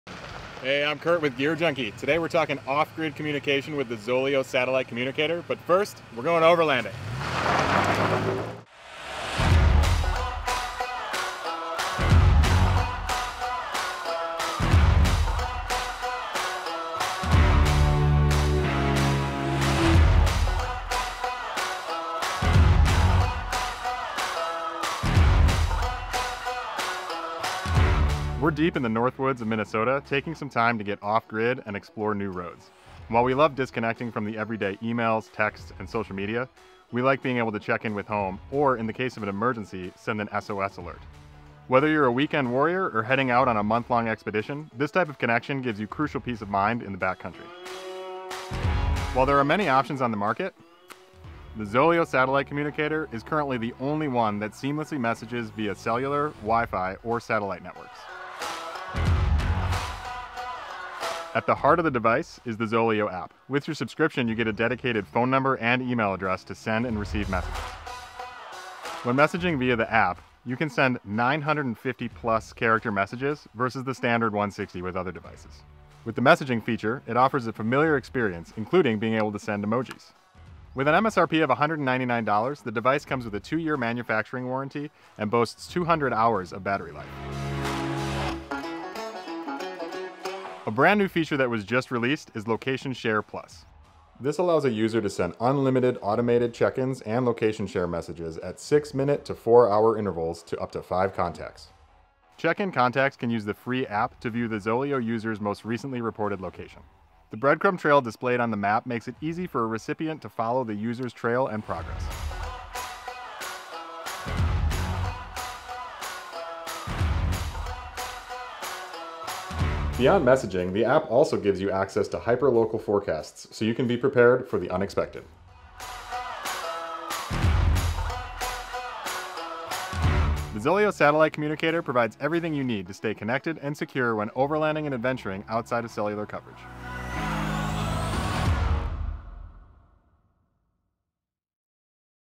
Off Grid Communication with ZOLEO
Planning for the unexpected while in the backcountry is a key part of any adventure. Whether you’re an experienced adventurer or it is your first time in the backcountry, the ability to connect with friends, family, and emergency services is paramount for safety and security while outside of cellular coverage.

Check out the video to learn more about ZOLEO’s satellite communicator and how it can keep you safe and connected while you’re off-grid.